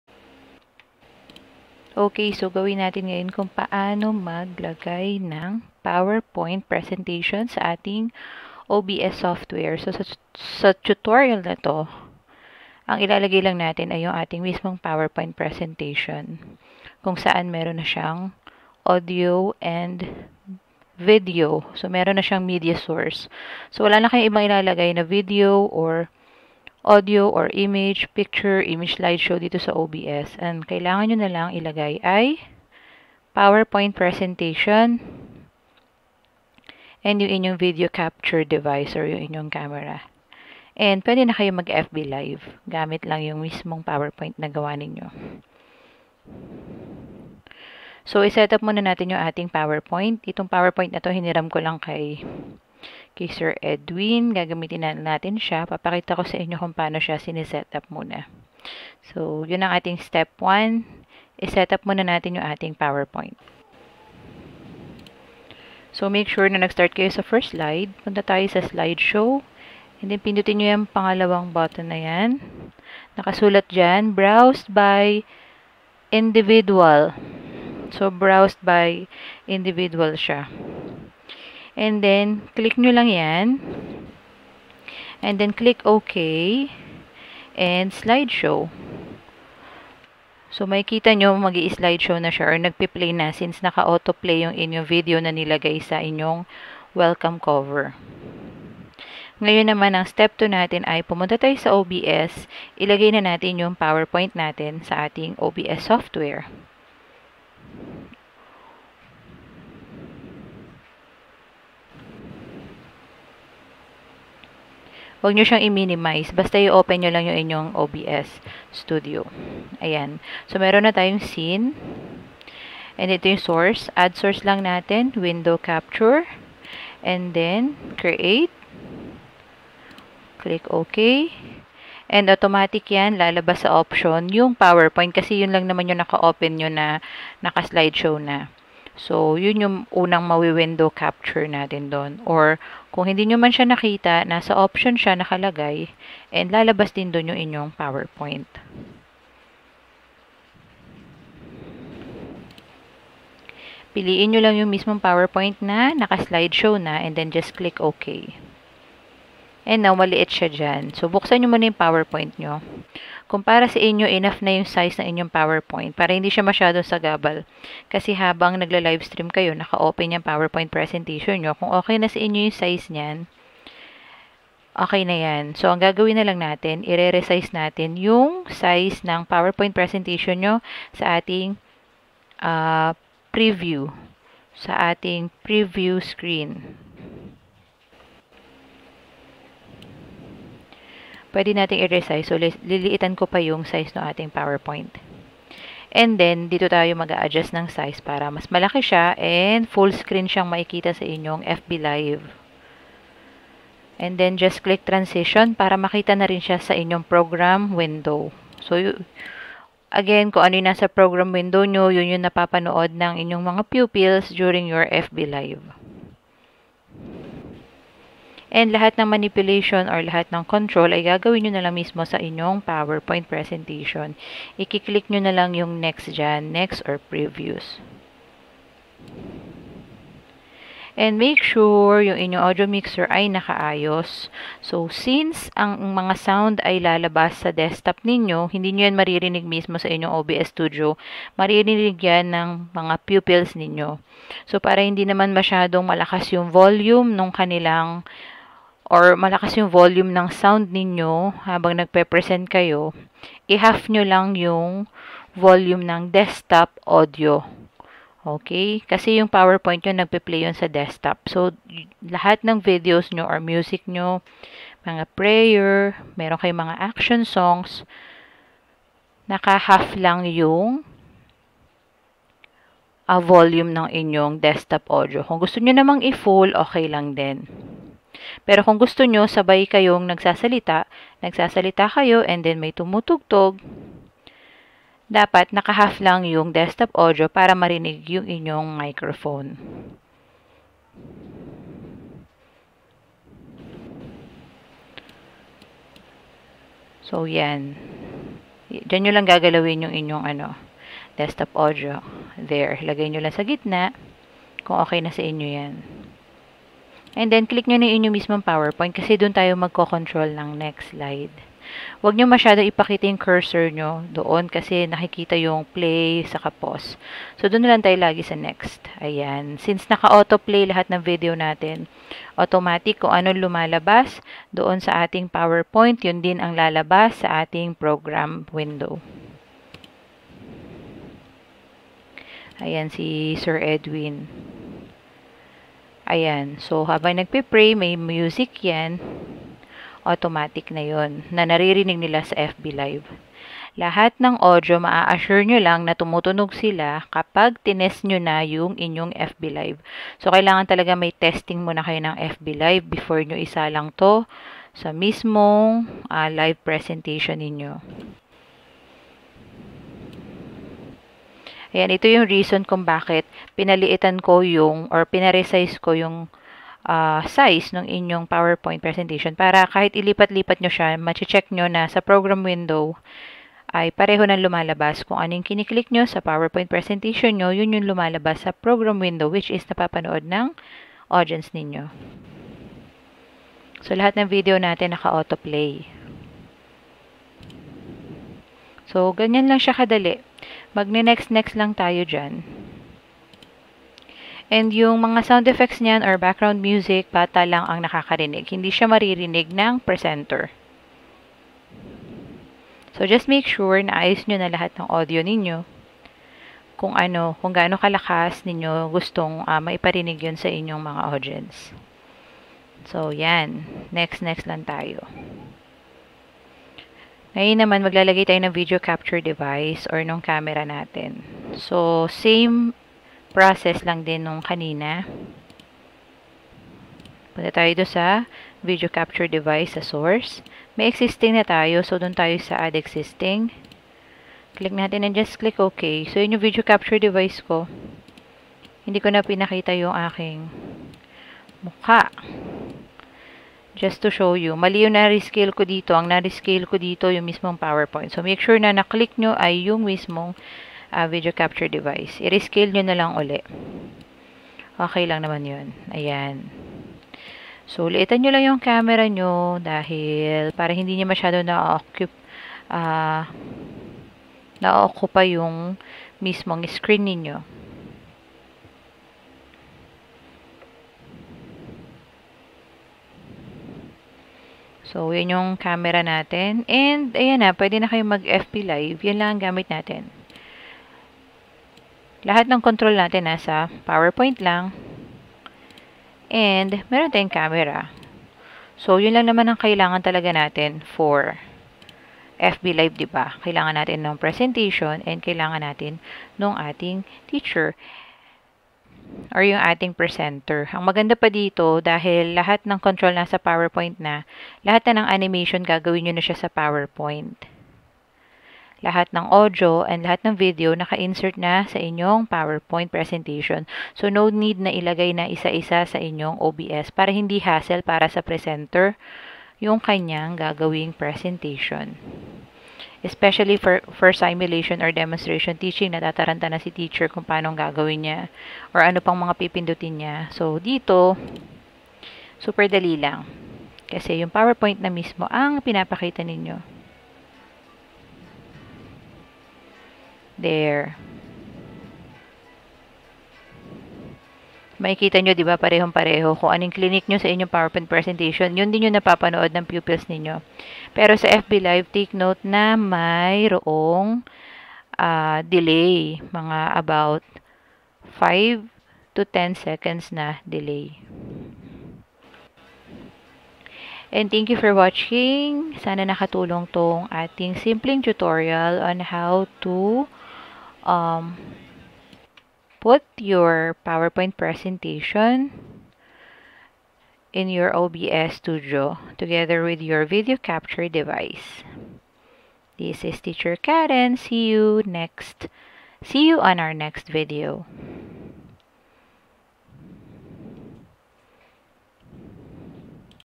POWERPOINT AND CAMERA LANG SA OBS STUDIO/ PWEDE NA SA FB LIVESTREAM
Hi, this is Teacher Khaye! Share ko lang gaano kadaling gamitin ang mismong PPT presentation sa OBS studio for your FB livestreaming :)